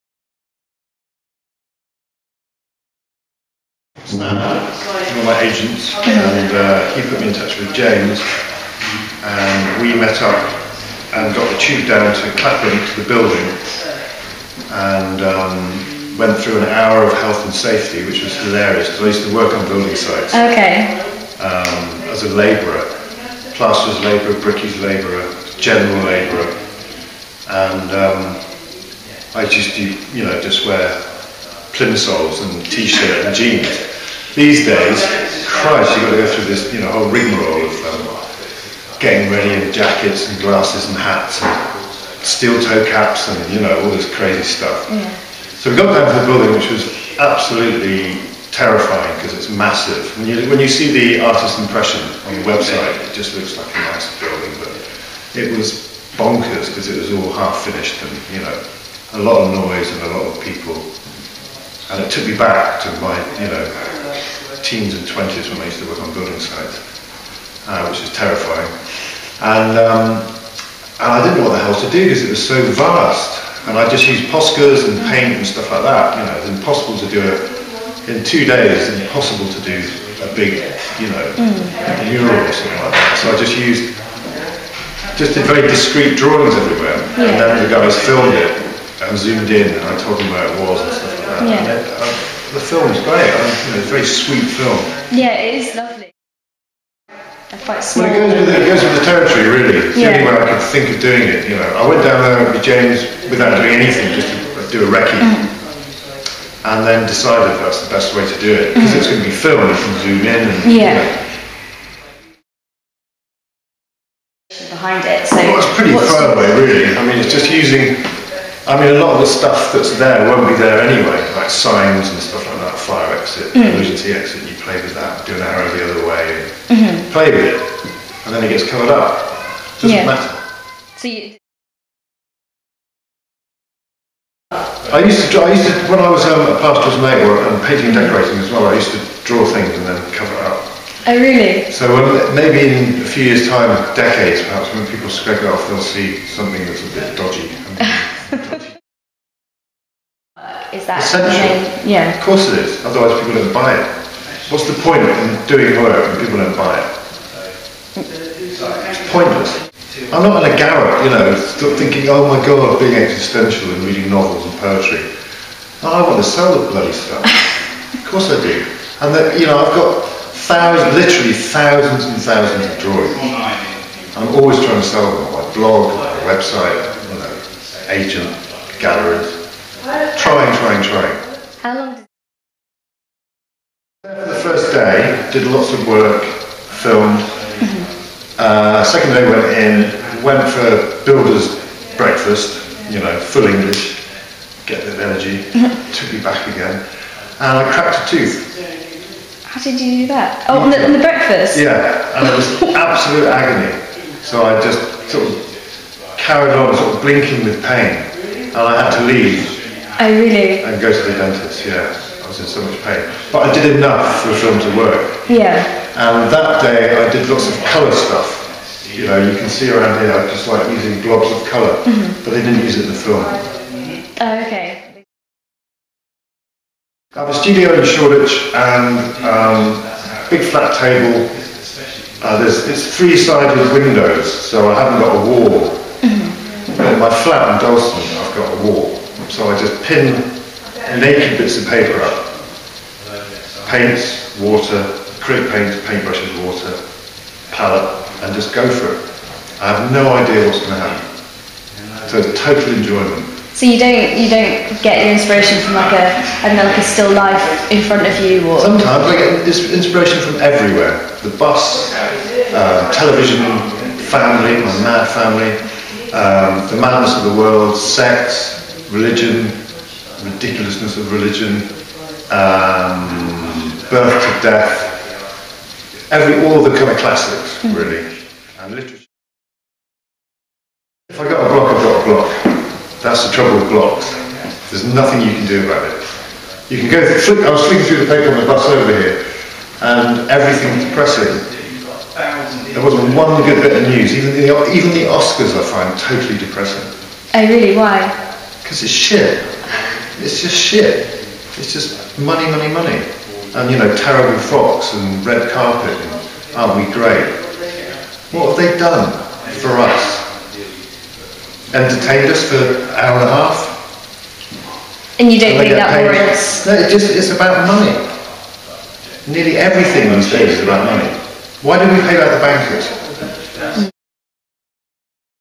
Interview - Paul Davis
XCity Arts & Culture interviews illustrator Paul Davis about his recent work on the Clapham Project